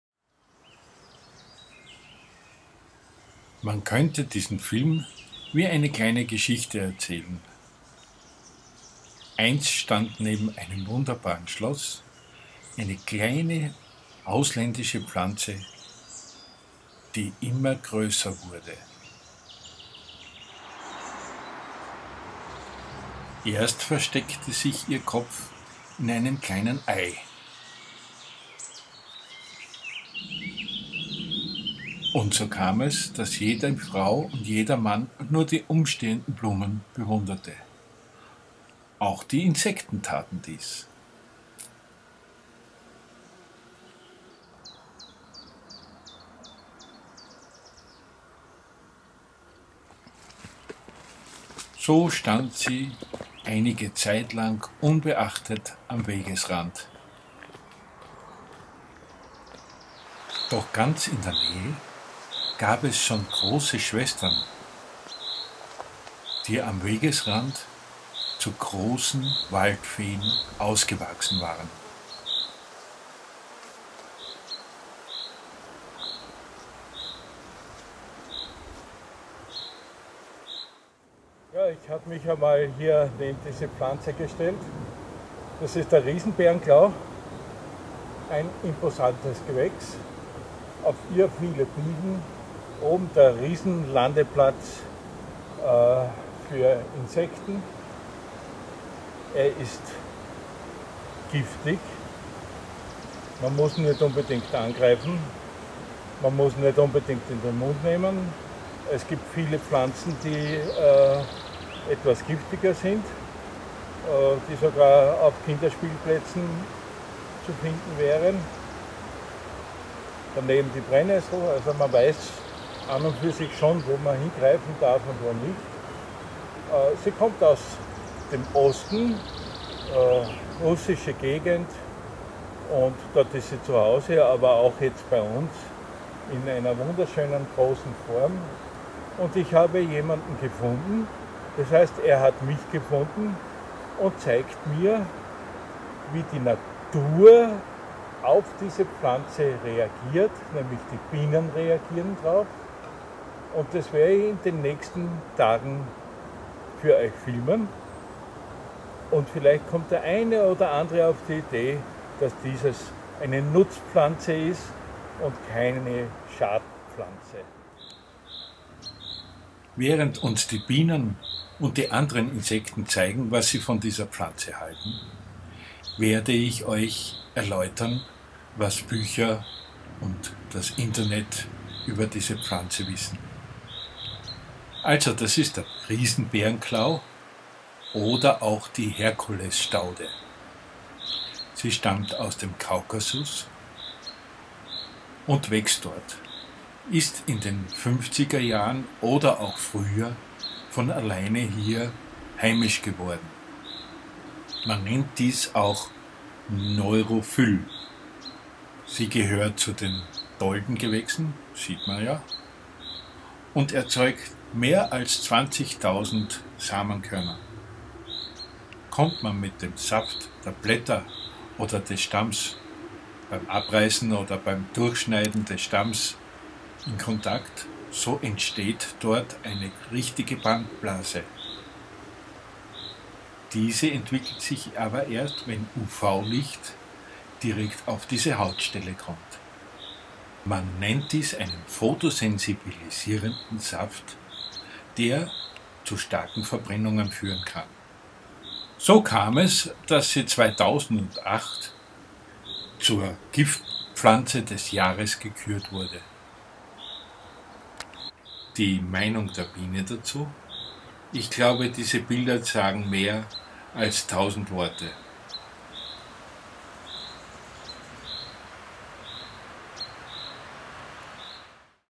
Die große weiße Fee 1
Mein neuer Film in HD Qualität. Die große weiße Fee 1 Teil:
Mein neuer Film Die große weiße Fee Handelt von der Herkulesstaude oder auch Berenklau  genannt, im Zusammenhang mit ihrer Bedeutung in der Natur und insbesondere den Bienen. Im Teil2 beschreibt Imker Johan Dokner seine Beobachtungen bezüglich der Bienenpopulation und der Herkulesstaude.